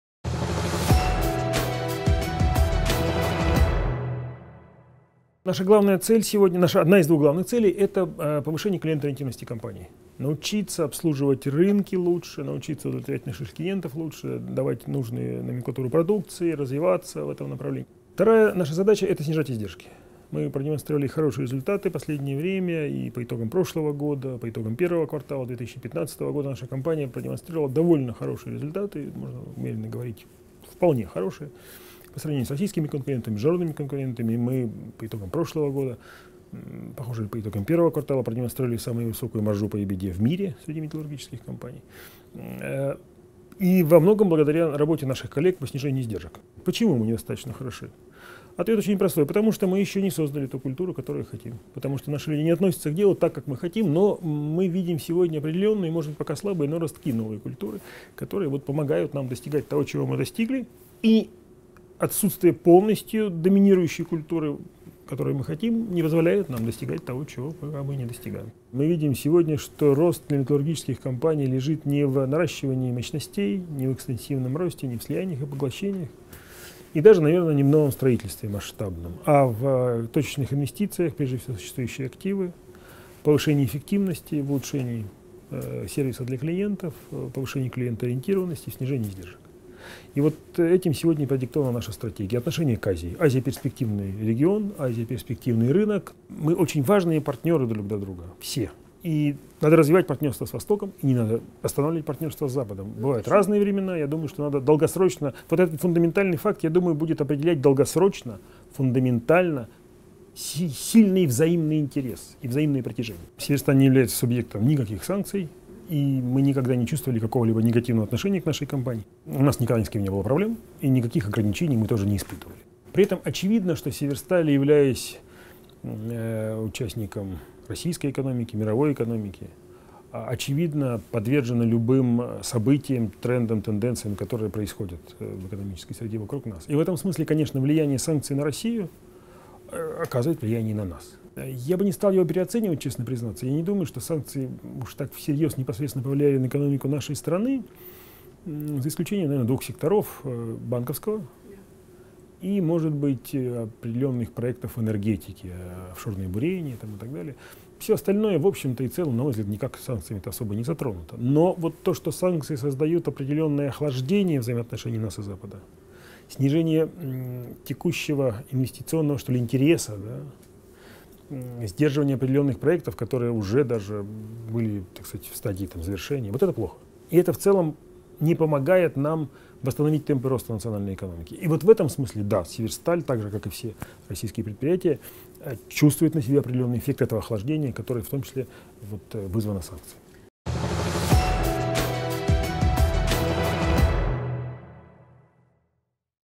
Alexey Mordashov, Chairman of the Board of Directors, PAO Severstal
Alexey Mordashov speaks about Severstal’s goals and strategy, as well as the impact of sanctions on the company as a part of the Russian economy.